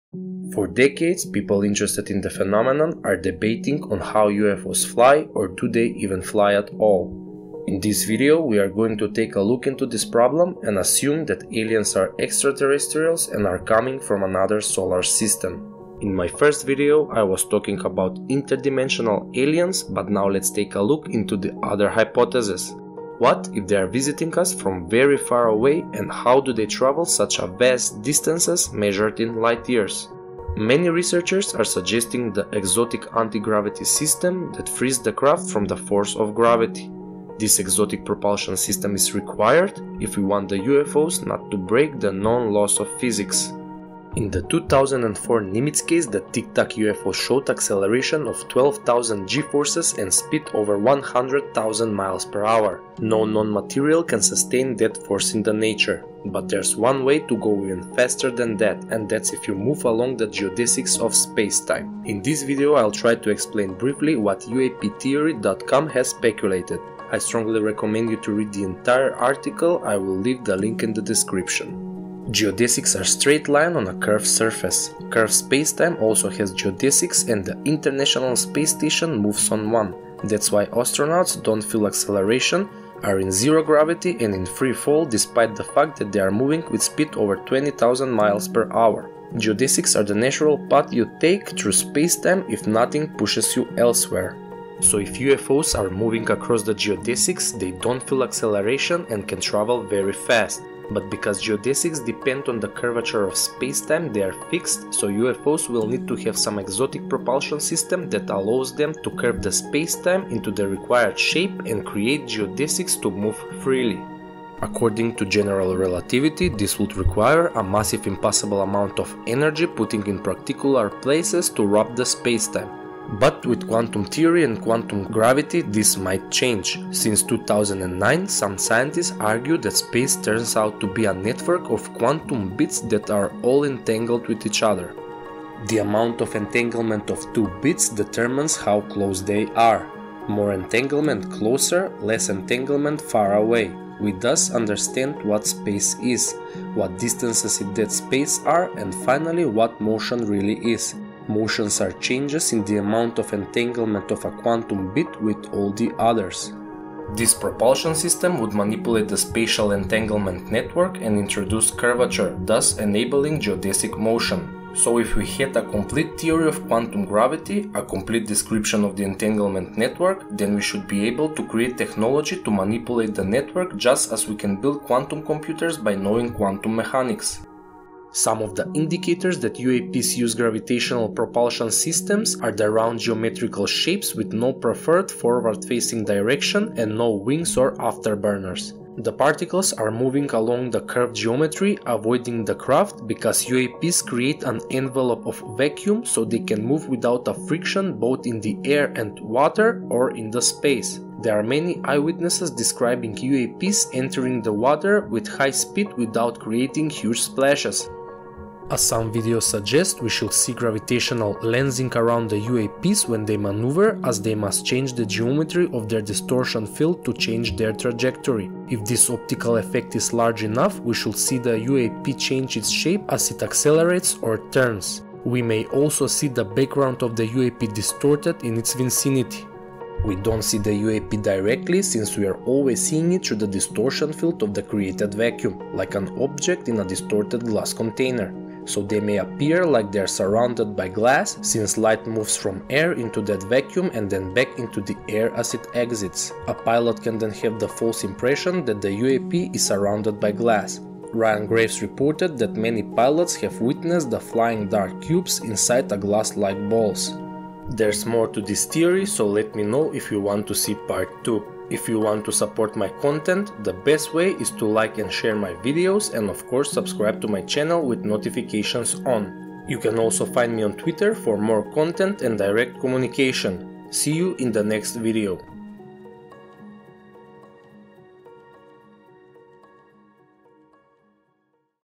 UAPs Propulsion System And How They Can Travel Faster Than The Speed Of Light
In this video I'm trying to describe the possible antigravitational propulsion system that is often mentioned and associated with the UAP/UFO phenomenon. Check out the uaptheory website for more details and let me know if you want part two